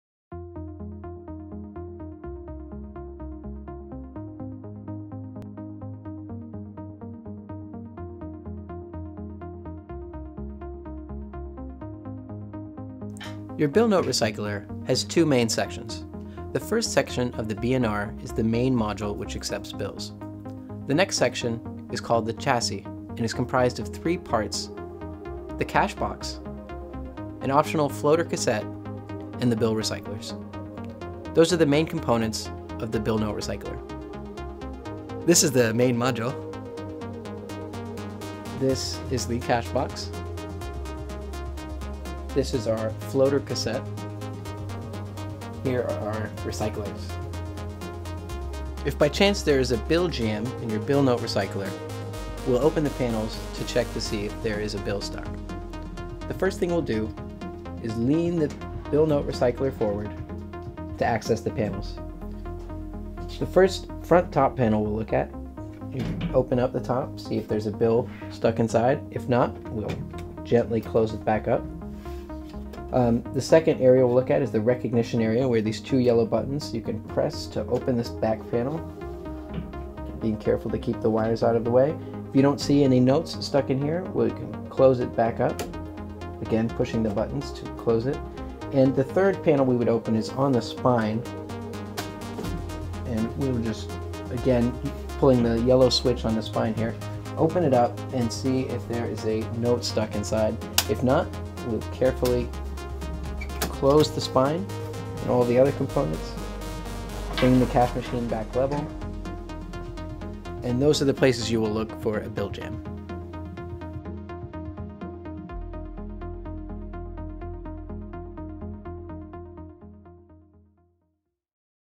Cash Machine: Troubleshooting Bill Jams on Your CM
Watch this demo video on how where to look for bill jams and the best way to clear them out.